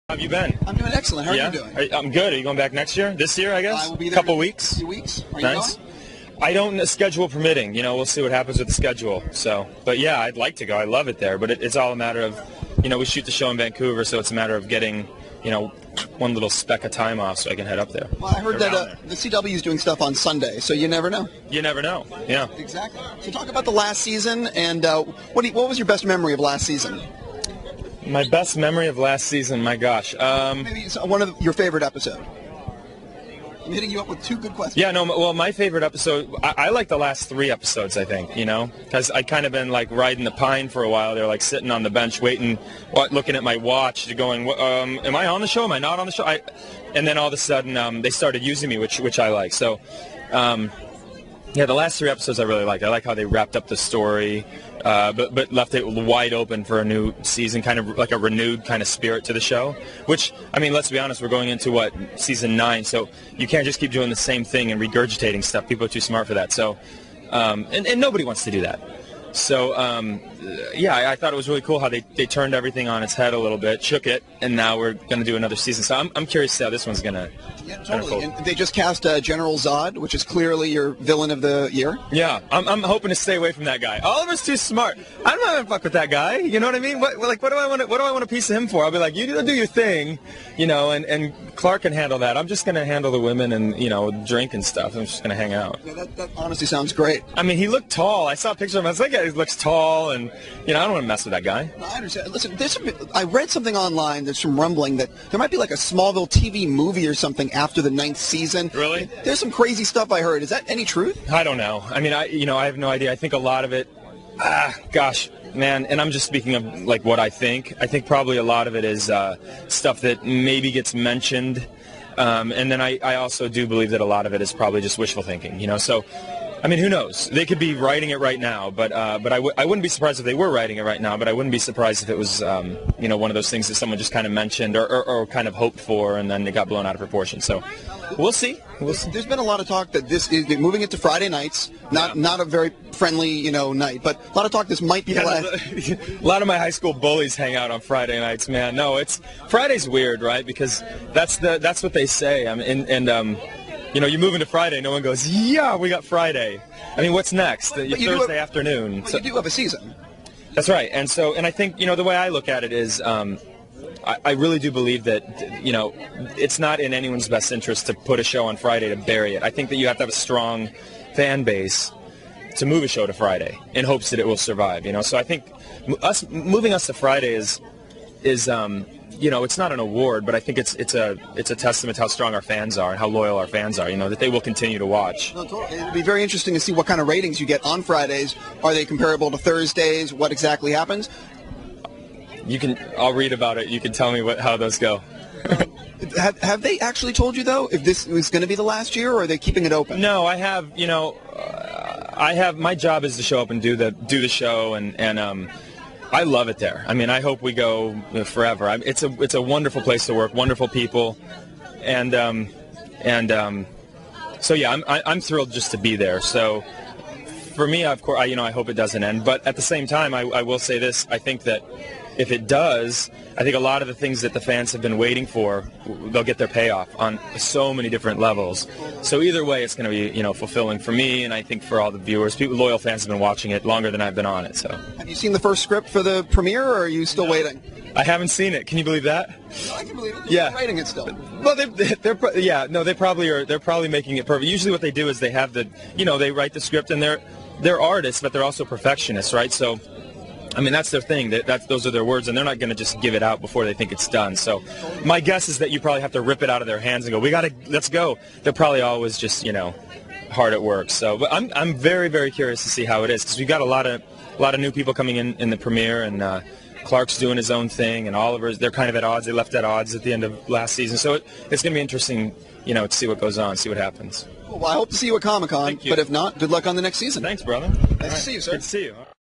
Justin Hartley Interview 2009 Saturn Awards
Justin Hartley (The Green Arrow on SMALLVILLE) Red Carpet Video Interview -- 2009 Saturn Awards Just for entertainment...
Thanks to Collider.com